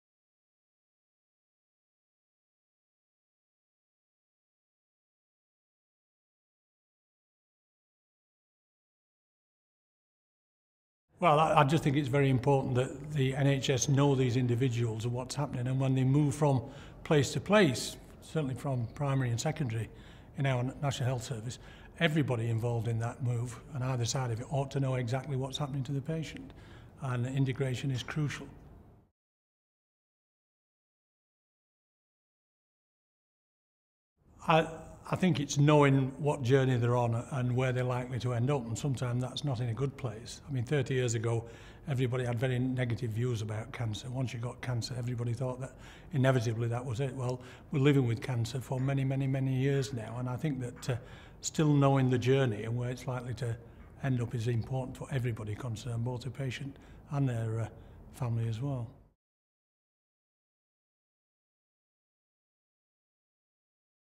Kevin Barron MP
Speaking about the importance of integrated cancer care at the Cancer Campaigning Group's parliamentary event on 20 June 2012.